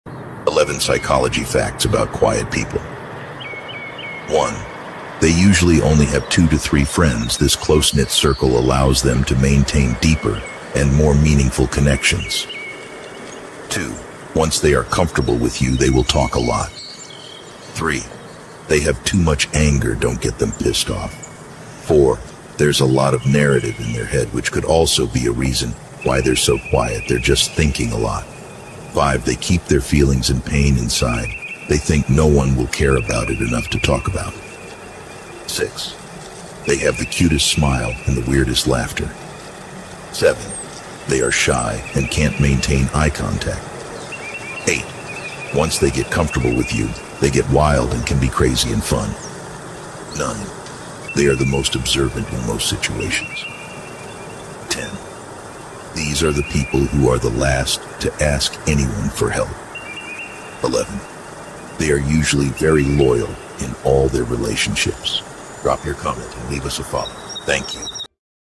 Unveiling the Psychology of Quiet People: 11 Fascinating Insights
Discover the intricate world of quiet individuals with these 11 psychology facts. From the depth of their thoughts to their preference for meaningful connections, delve into the unique characteristics that shape the lives of quiet people. Gain a deeper understanding of introversion, creativity, and emotional intelligence, and explore how these traits contribute to a rich and fulfilling inner world.

QuietPeople